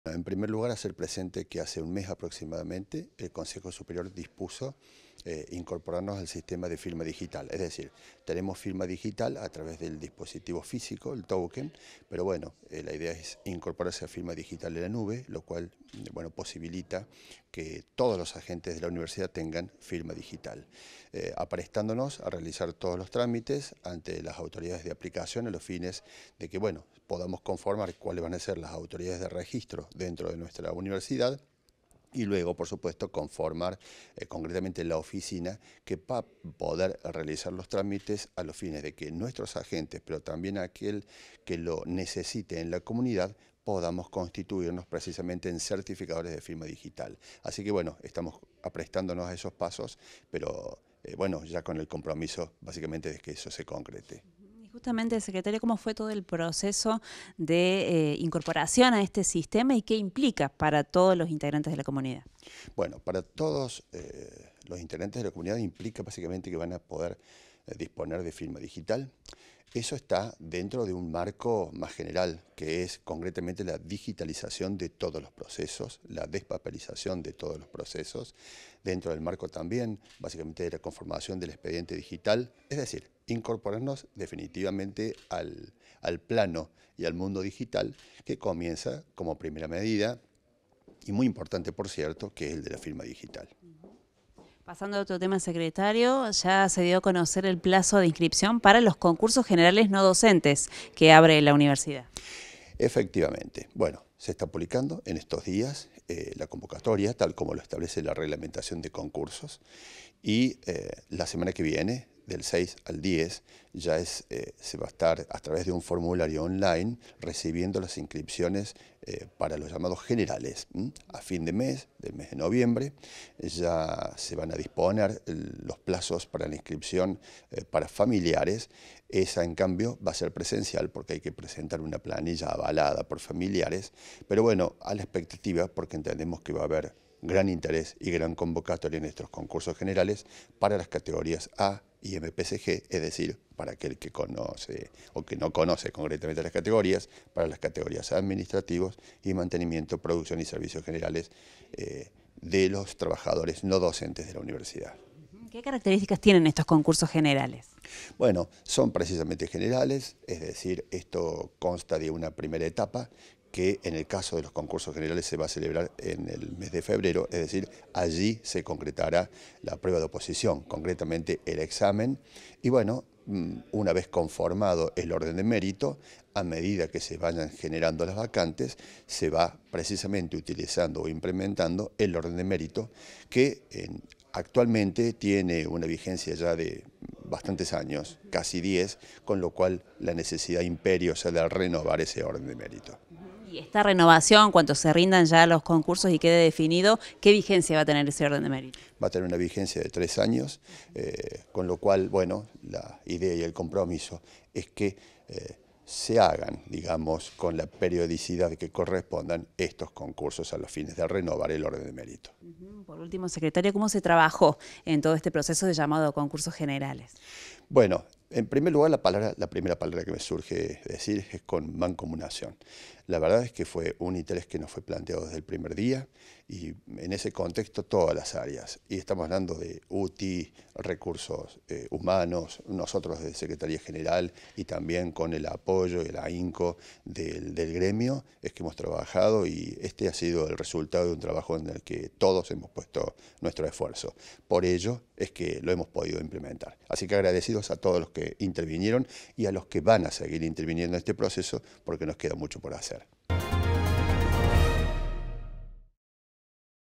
LA UNRC SE INTEGRA A LA PLATAFORMA DE FIRMA DIGITAL REMOTA. EL PROYECTO FUE IMPULSADO DESDE LA UTI
JORGE MIGUEL – SECRETARIO GENERAL
UniRio TV
Universidad Nacional de Río Cuarto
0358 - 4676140